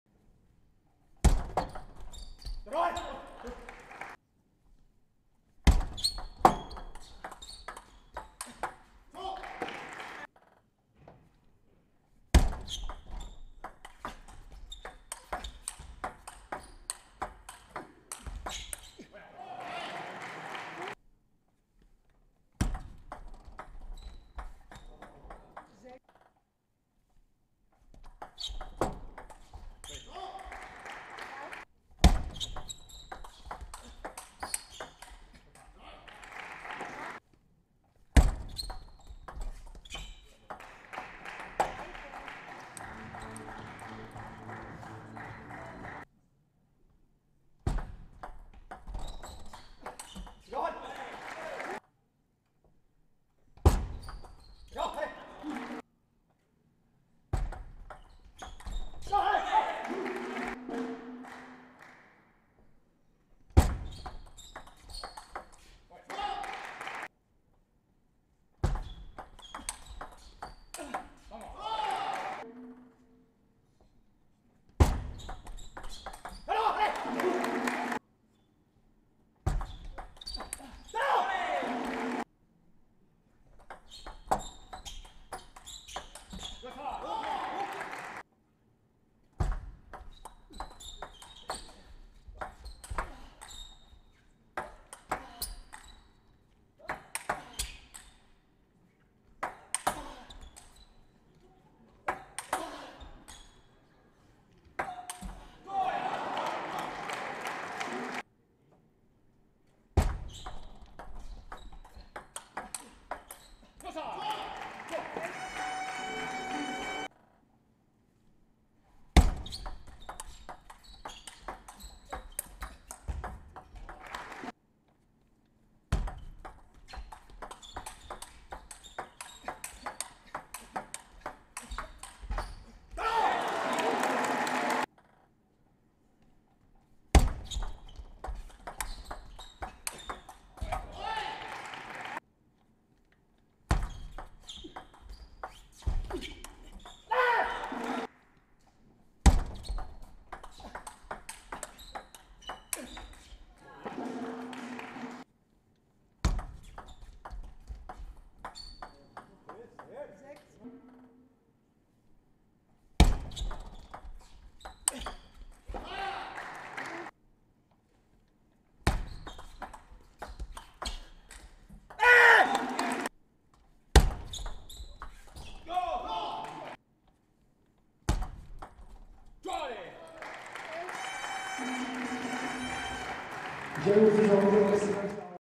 Marcelo Aguirre vs Bastian Steger (TTBL Selected) I Saison 2022/23
Saison 2022/23  - 15. Spieltag - SV Werder Bremen vs TSV Bad Königshofen